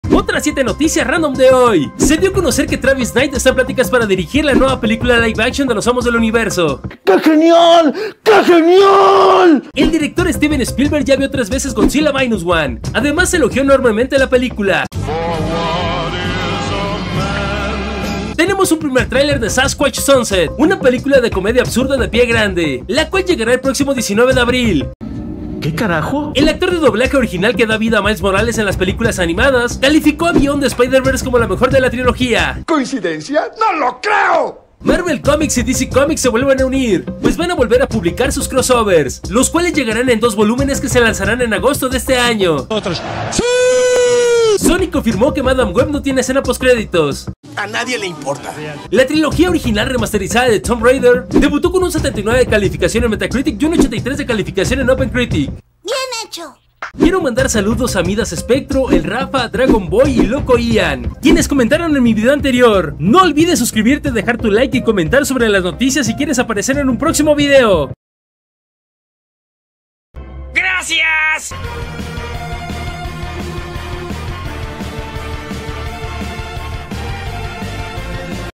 OTRAS 7 NOTICIAS RANDOM | MARVEL Y DC JUNTOS DE NUEVO, HE-MAN LIVE-ACTION, PIE GRANDE PELICULA Y MÁS
Otras 7 noticias random de hoy.

noticias ultimahora videojuegos películas cine otrasnoticiasrandom noticiasrandom